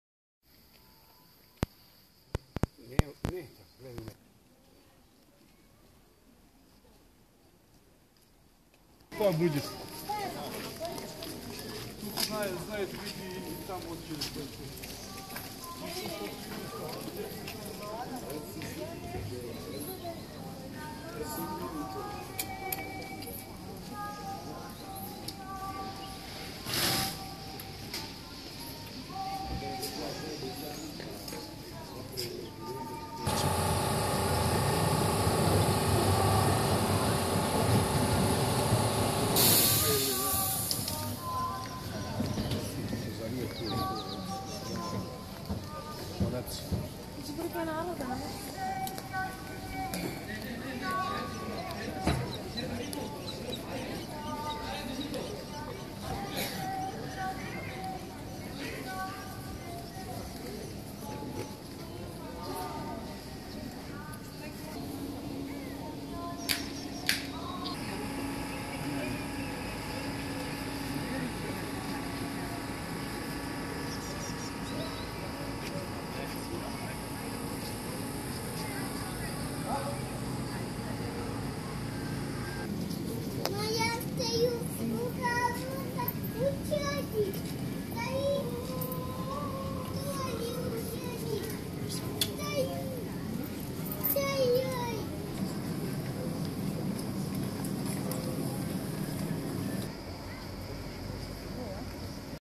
Monastery Ostrog, Montenegro - Ostrog Manastiri Karadag
Sumela Manastirina benzeyen yapisı, sarp kayaliğa kurulmuş kartal yuvasını amdırması, gizli gecitleri ve tahrip olmamis yapisiyla ziyaretcilerini hayran birakiyor.
17. yy yapılan manastır 1923 de restore edilerek bugunki halini almıştır.